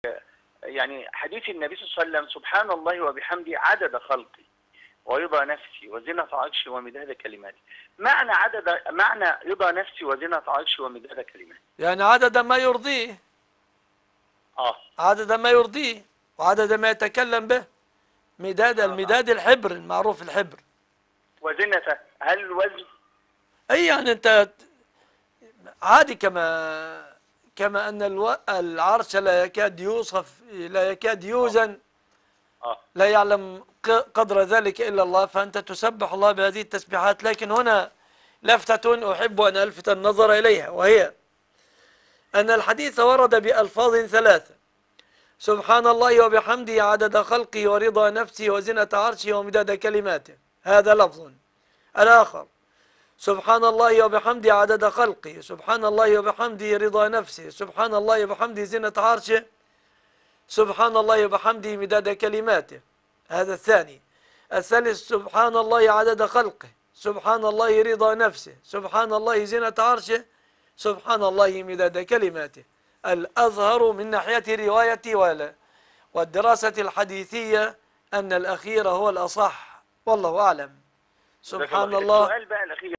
ما معني رضا نفسه وزنة عرشه ومداد كلماته | الشيخ مصطفي العدوي
ما معني رضا نفسه وزنة عرشه ومداد كلماته | الشيخ مصطفي العدوي  تقدر تكون إيجابي لو أعجبك المقطع وتعمل اعجاب 👍 وأعادة نشر📢  ويا سلام لو تعمل تعليق ✉